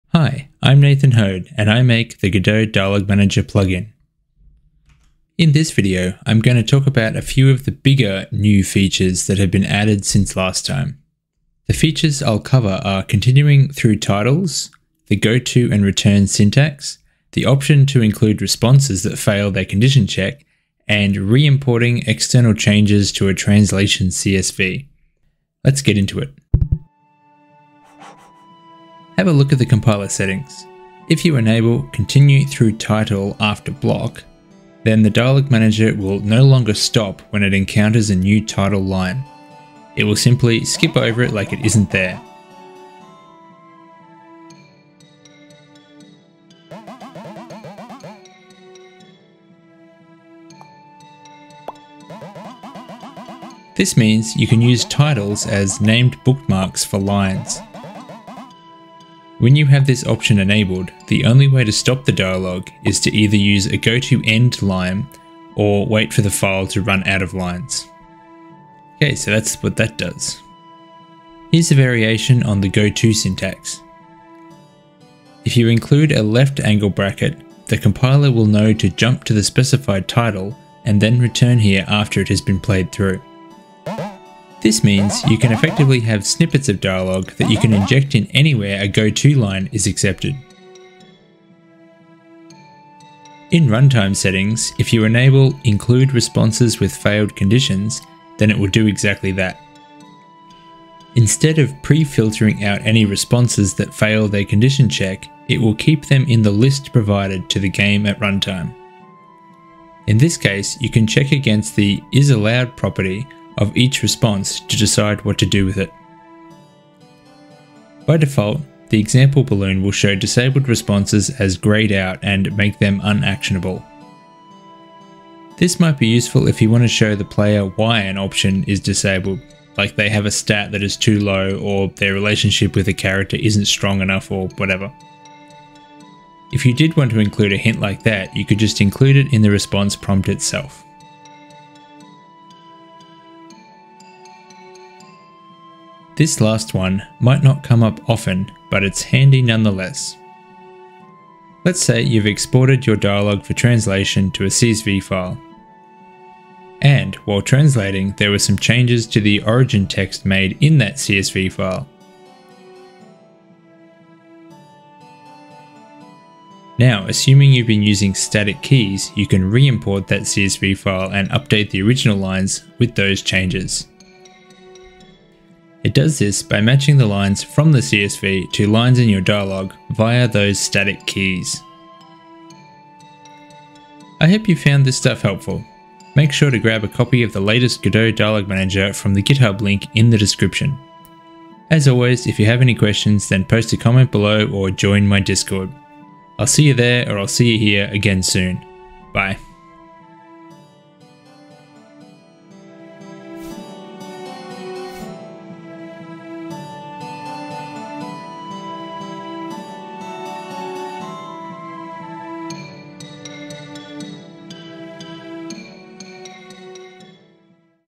New Features for Dialogue Manager for Godot 3
In this video I cover a few of the new features that have been recently added to my Godot Dialogue Manager addon.

00:28 Continuing through title setting
01:10 Snippets
01:33 Include failed responses setting
02:25 Re-importing modified translation CSVs